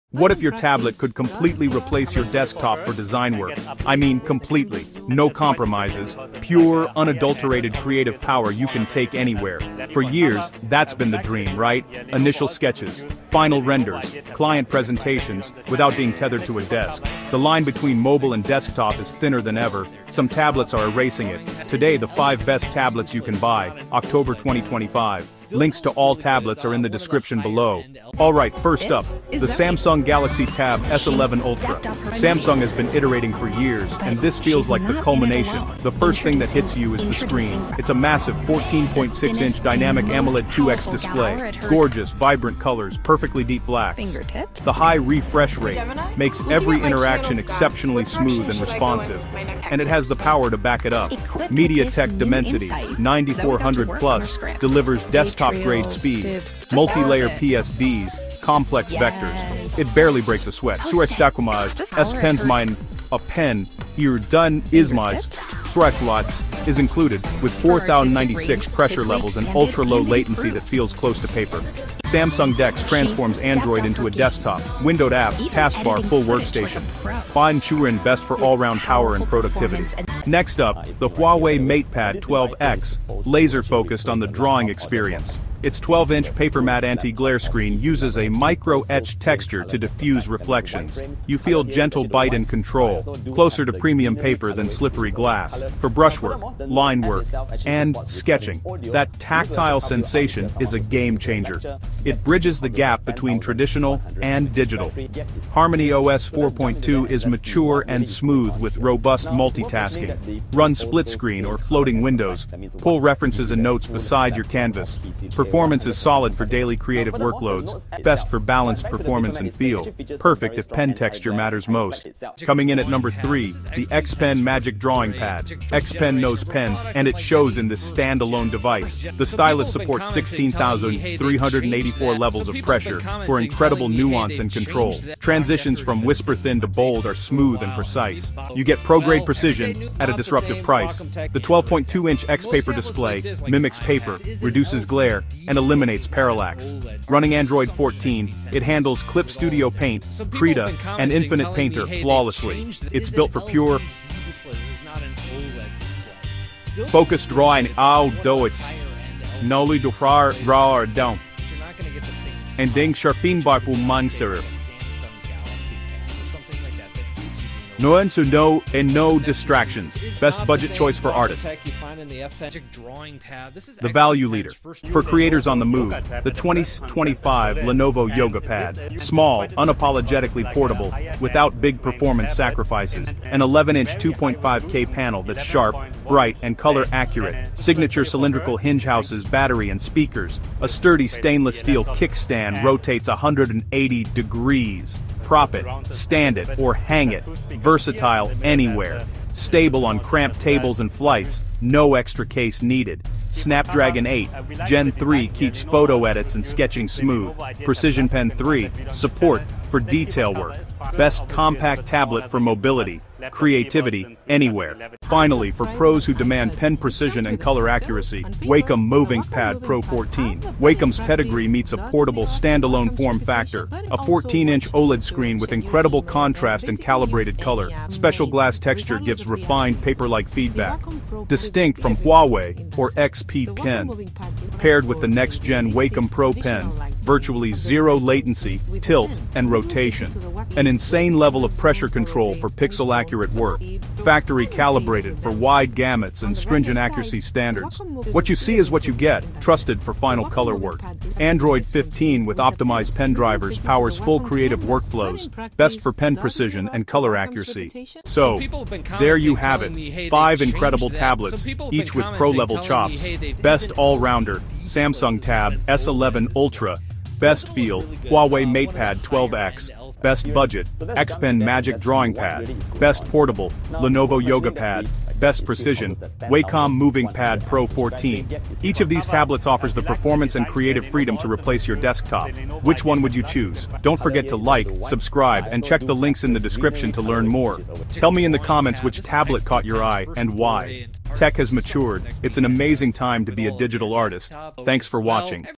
Ditch Your PC For These 5 AMAZING Pro Tablets That Do It All Now
Here Are The Tablets
1. Samsung Galaxy Tab S11 Ultra
2. Huawei MatePad 12 X
3. XPPEN Magic
4. 2025 Lenovo Yoga Pad
5. Wacom MovinkPad Pro 14

What if your tablet could completely replace your desktop for design work? In this fast, tech‑inspired 5‑minute roundup we test 5 pro tablets you can use for serious digital art and productivity — Samsung Galaxy Tab S11 Ultra, Huawei MatePad 12 X, XPPEN Magic Drawing Pad, Lenovo Yoga Pad (11"), and Wacom MovinkPad Pro 14. Expect upbeat stock music, modern motion graphics, text overlays, smooth transitions, and clear spec callouts. Links to all tablets and gear are in the description below. Keywords: tablets for designers, pro tablets, replace desktop, digital art, graphic design, pen display.

OUTLINE:

00:00:00 Intro - The New Era of Design
00:00:28 Tablet #1 - Samsung Galaxy Tab S11 Ultra
00:01:20 Tablet #2 - Huawei MatePad 12 X
00:02:02 Tablet #3 – XPPEN Magic Drawing Pad
00:02:54 Tablet #4 – Lenovo Yoga Pad (11-inch, 2025)
00:03:34 Tablet #5 – Wacom MovinkPad Pro 14
00:04:17 Summary & Outro